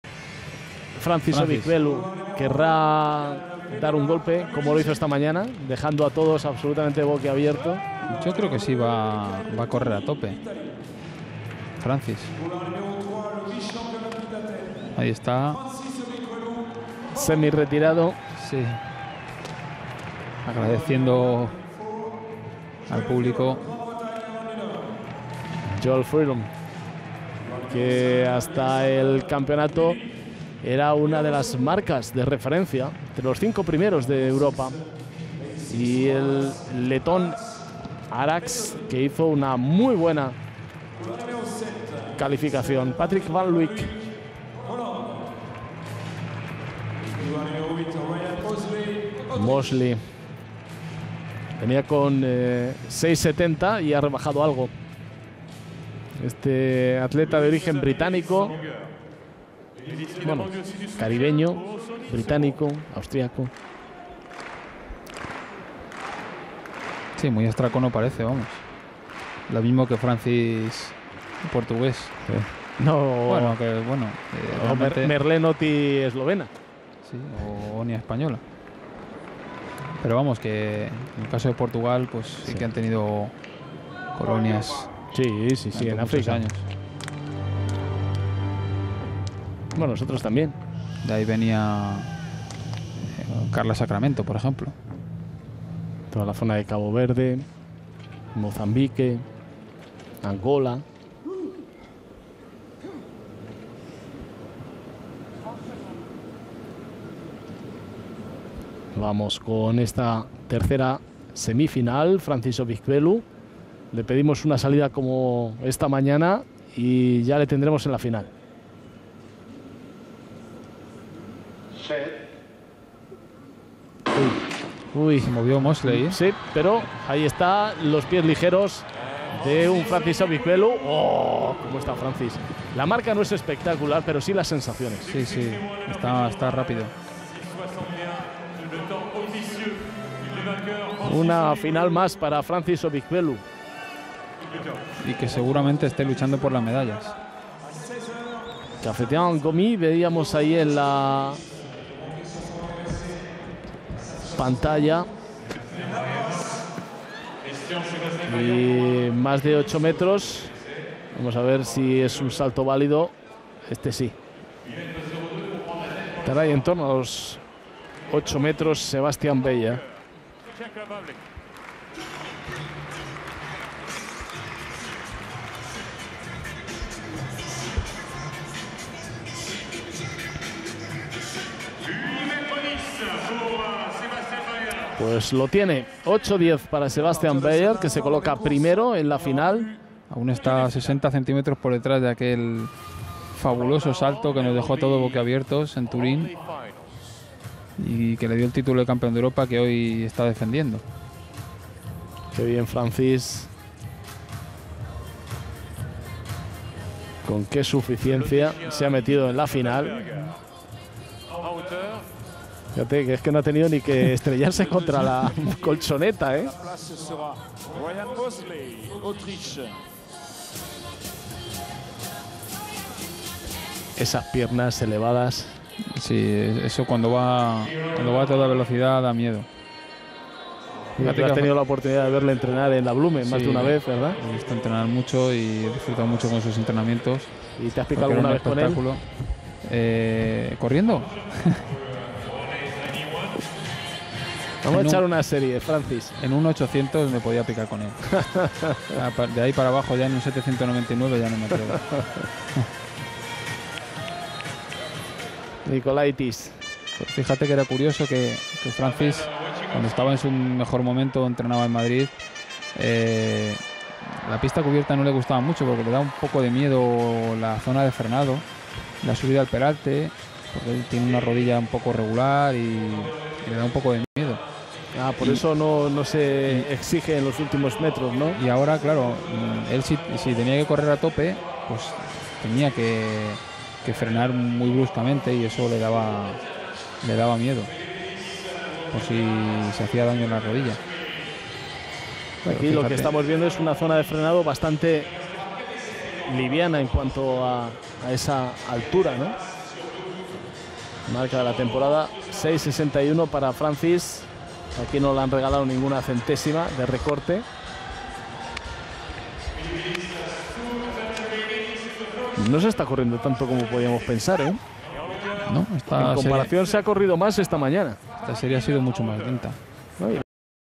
Atletismo Cto Europa Pista Cubierta 60m semifinal 3
Rank   Athlete Nation Result
1    OBIKWELU, Francis  POR 6.61 Q  =SB
2    MOSELEY, Ryan  AUT 6.68 Q  SB
3    FEARON, Joel  GBR 6.69
4    VAN LUIJK, Patrick  NED 6.70
5    VELEBA, Jan  CZE 6.71
6    LINNIK, Aliaksandr  BLR 6.71
7    TUMI, Michael  ITA 6.71    =PB
8    ARAJS, Ronalds  LAT 6.78